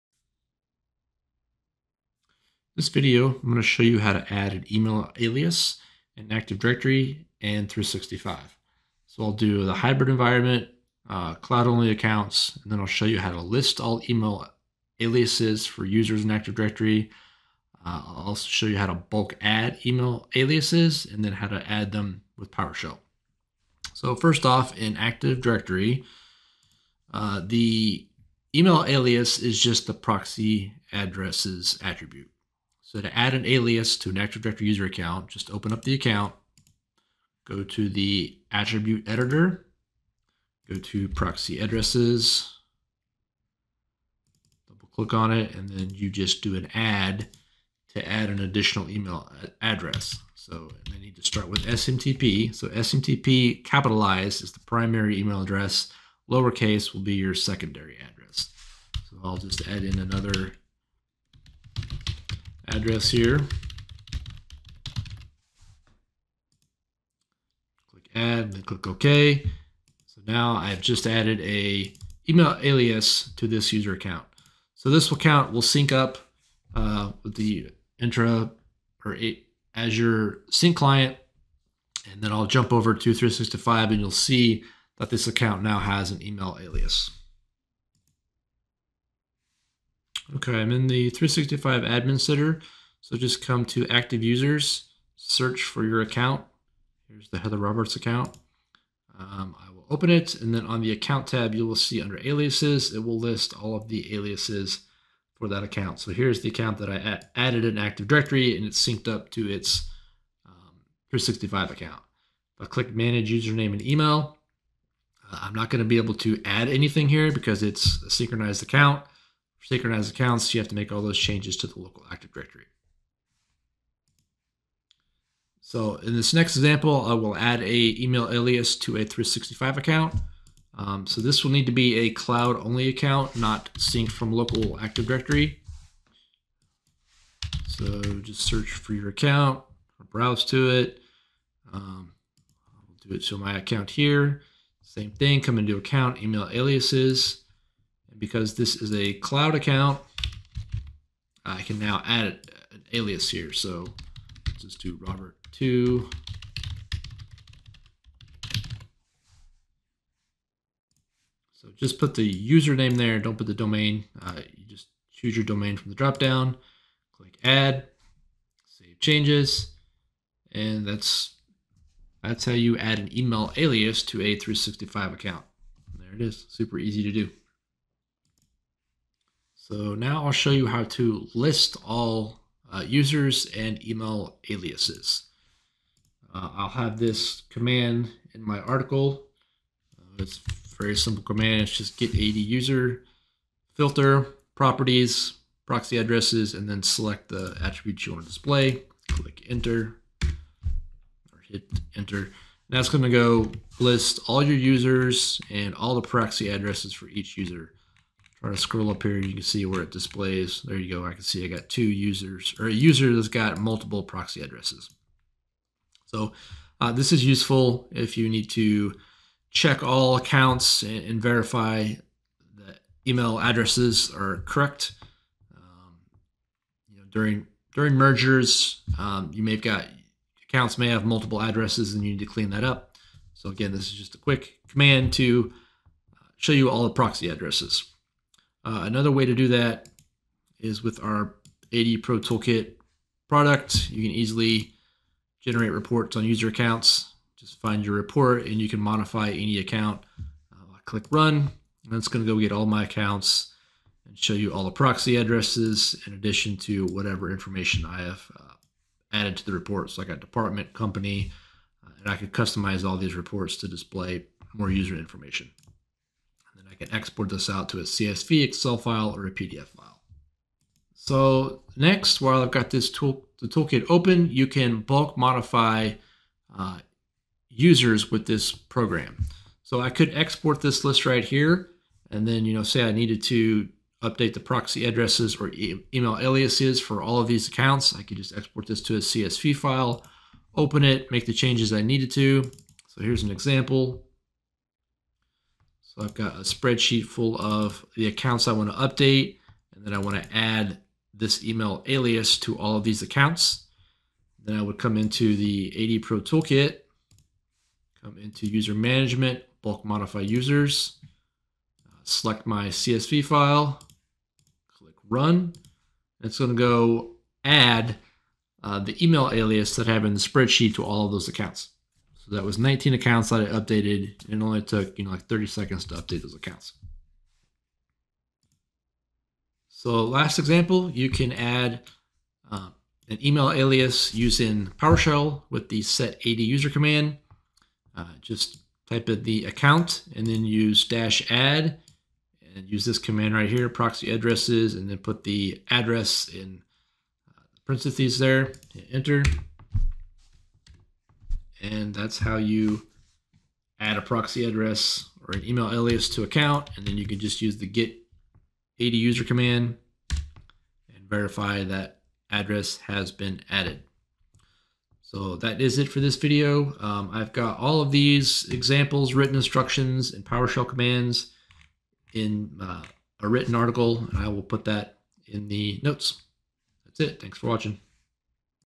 How to add email alias in active directory
Video on how to add an email alias to Active Directory and Microsoft 365.